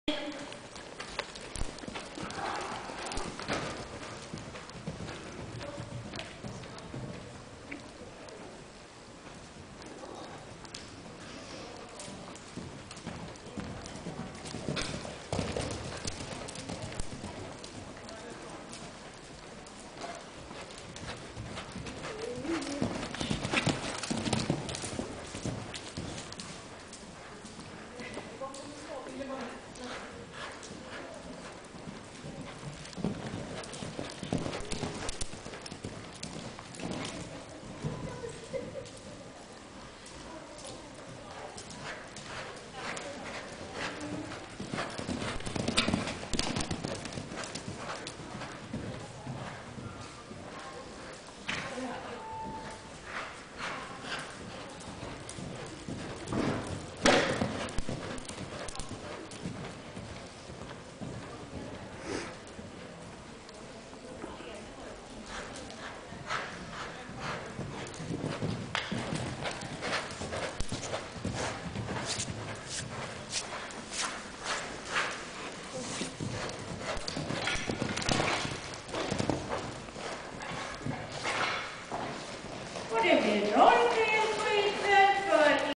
tavling-090118-60cm-elin-bonza
Elins första tävling med mammas häst Bonanza.
18 januari 2009 på klubben.
60 cm. Gick kanonbra och fick rosett.